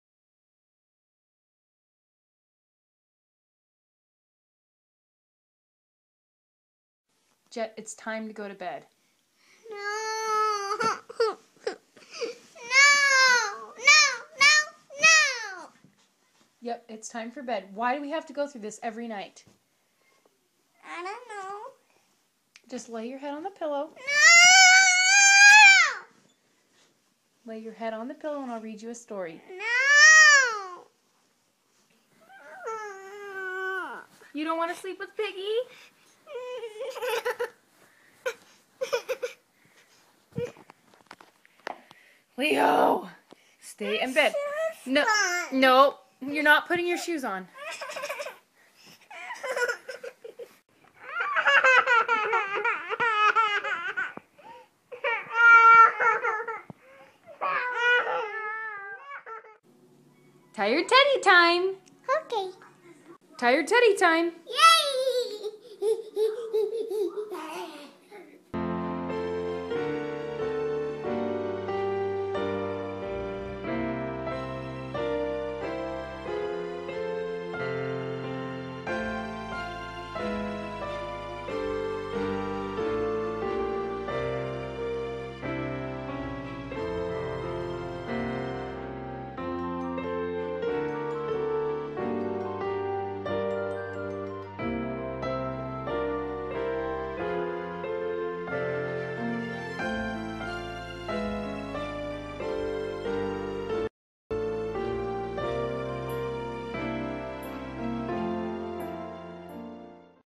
Bedtime Struggles: A Video Diary
Parents reenact their kids' bedtime tantrums for Tired Teddy video contest!